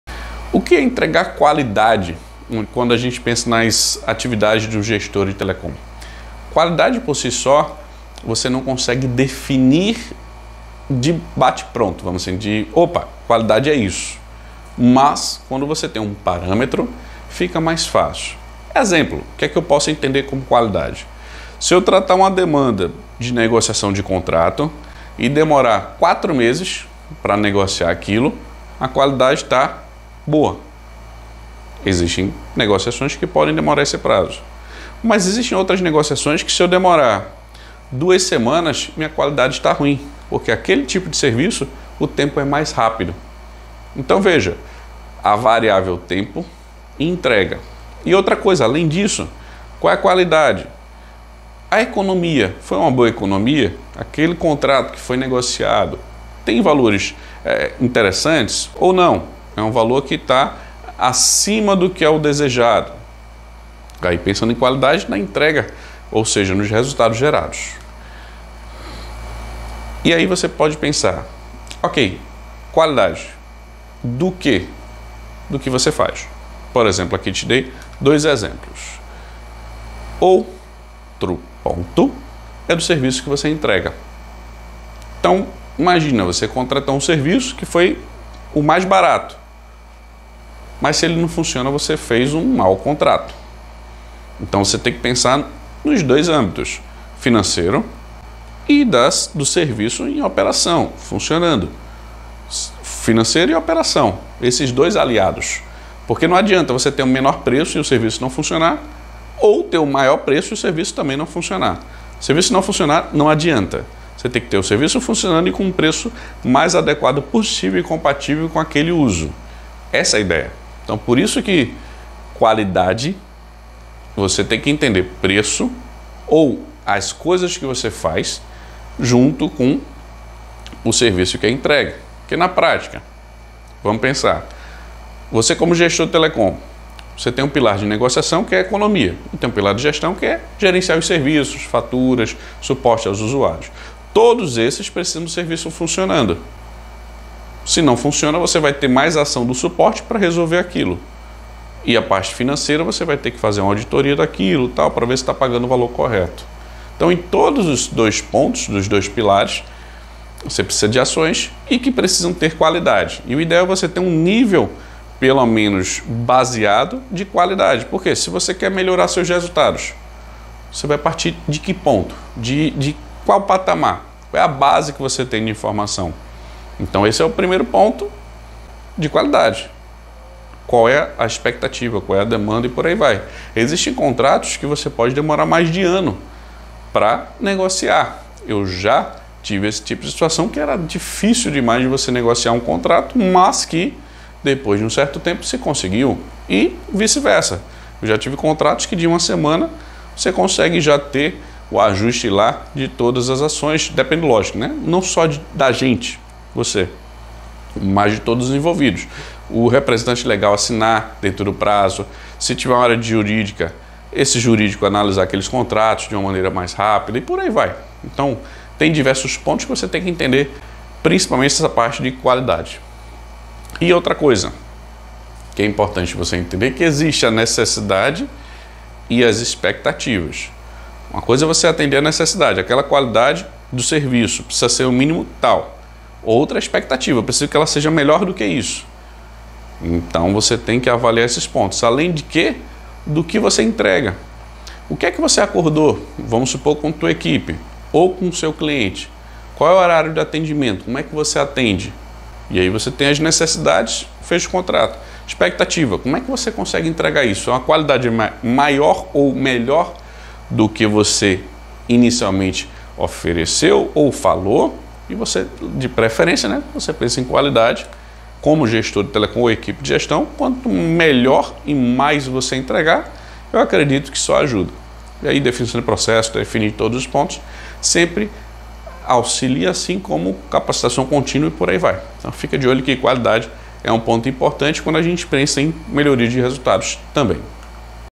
O que é entregar qualidade nas atividades de um gestor de telecom
Neste vídeo te explico o que é entregar qualidade nas atividades de um gestor de telecom, seja do que você faz ou do que o serviço entrega e sempre de olho em entregar o melhor resultado possível.

Dúvidas?
Manda um comentário...

Até+